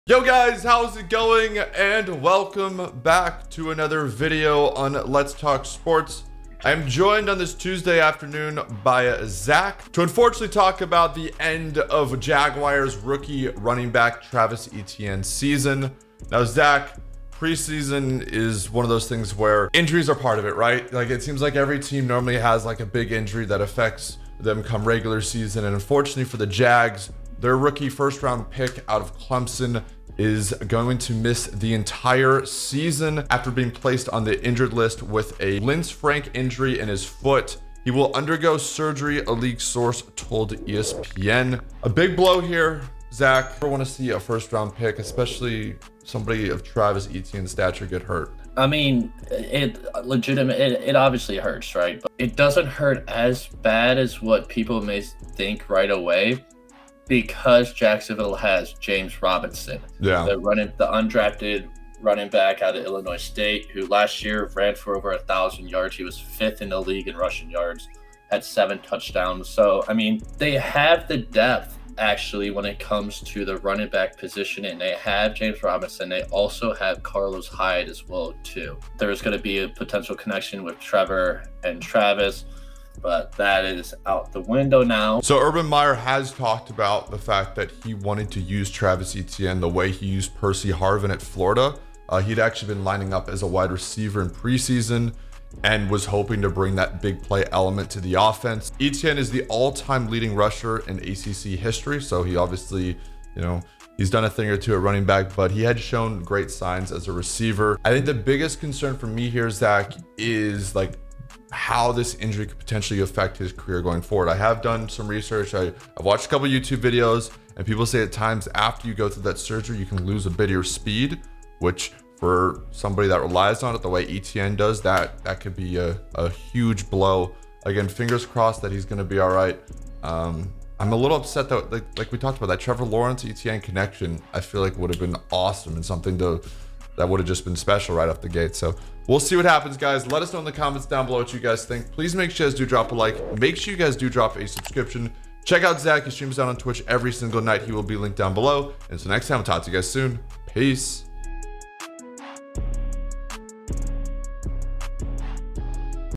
Travis Etienne Season Ending Foot Injury
The Jacksonville Jaguars have lost one of their 2021 first-round picks, Travis Etienne will undergo foot surgery.